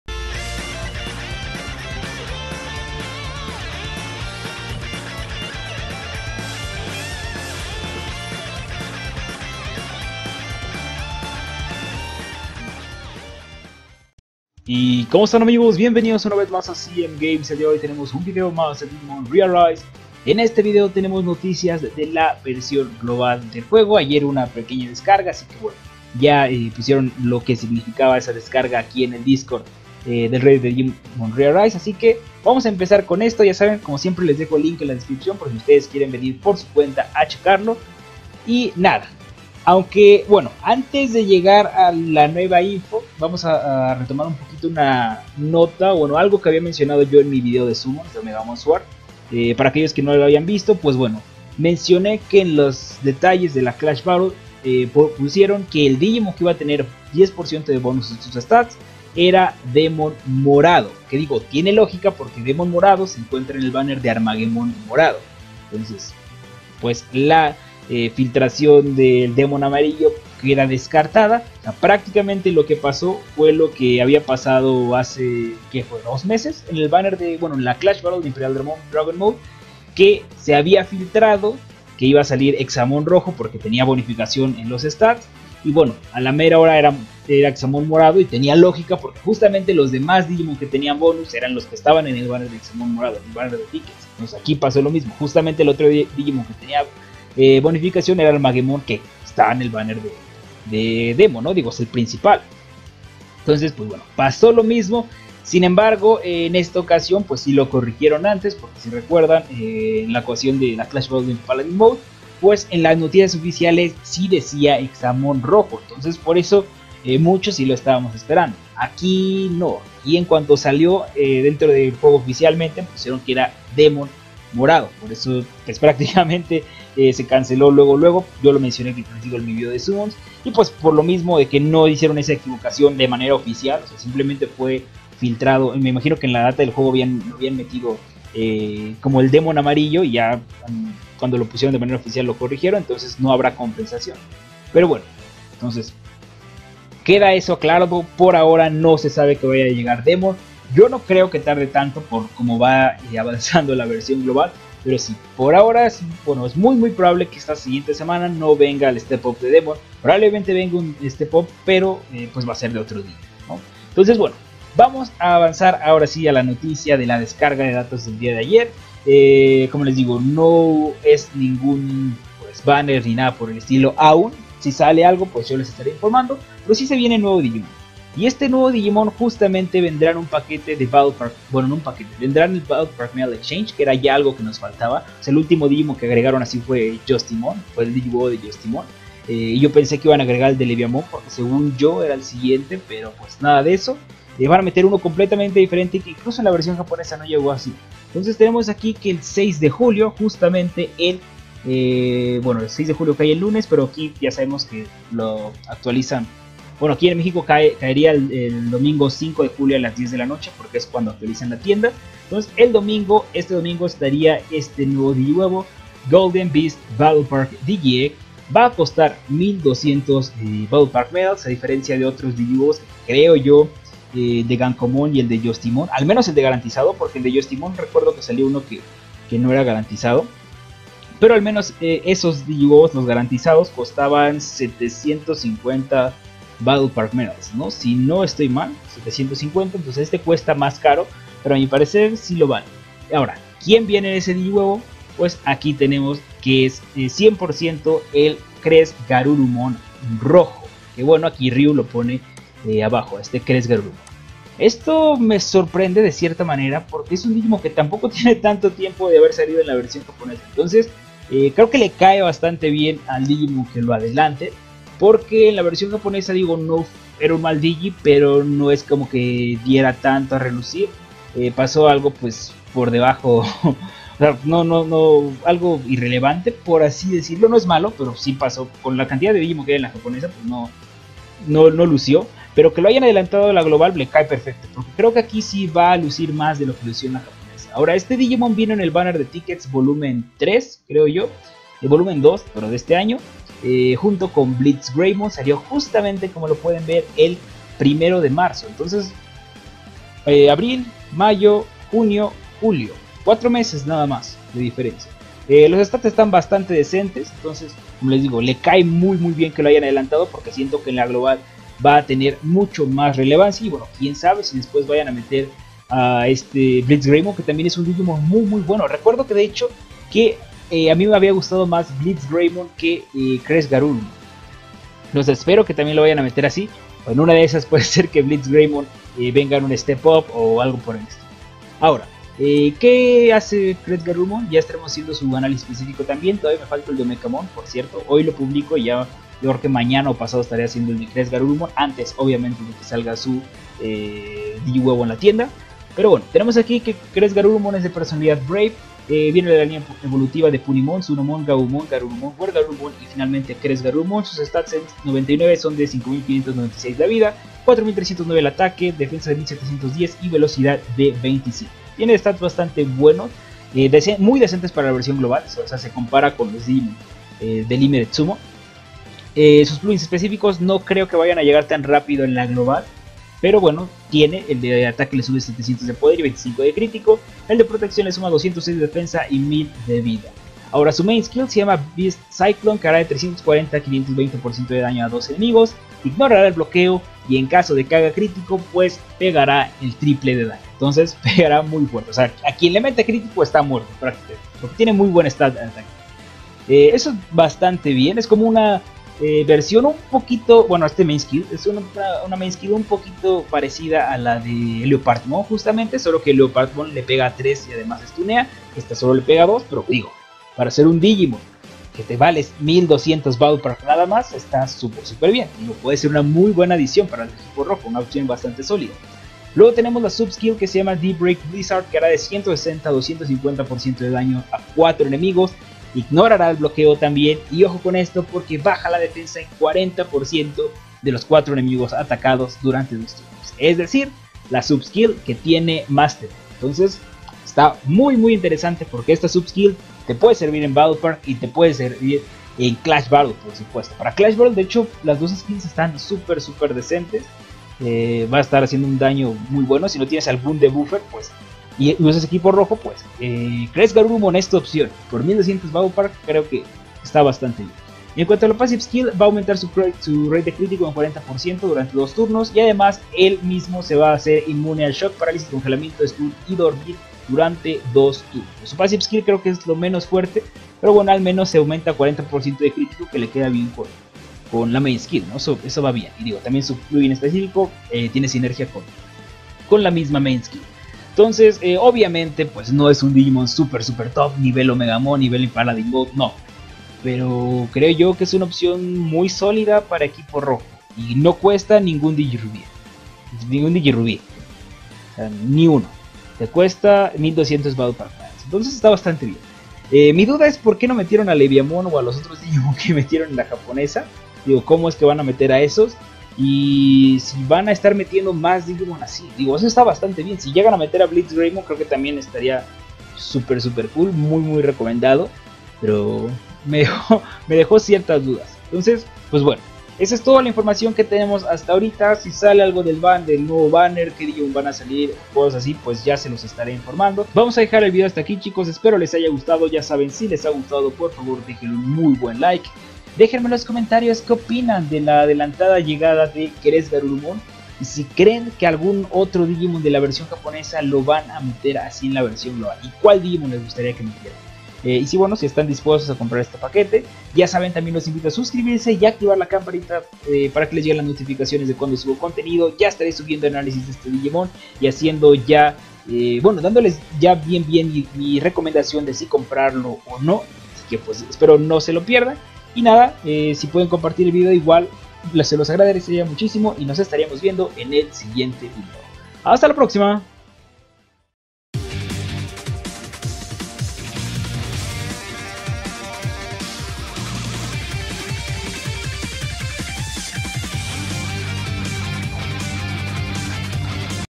¿OTRO DIGIMON ADELANTADO? Noticias Digimon ReArise
Un nuevo Digimon está por llegar a la versión Global de Digimon ReArise y en este vídeo veremos de que se trata.⬇️⬇️⬇️⬇️⬇️

⛔Siganme en Twitter, ahí pueden enviarme preguntas o dudas que tengan del juego o pueden compartir sus logros.⛔

⚠️Agradezco a Riu y Chortos por compartir la información.⚠️